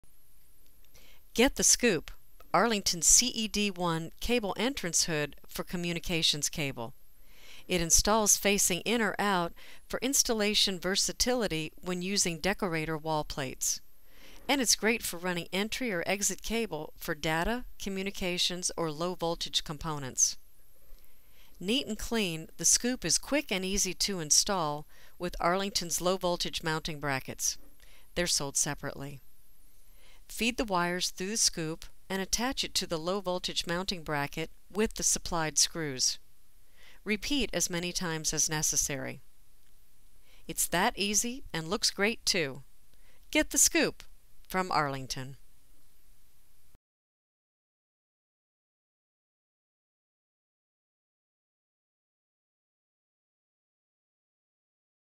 Arlington's "The Scoop(tm)" Line of Products
A demonstration of Arlington's "The Scoop(tm)" entrance plates and entrance hoods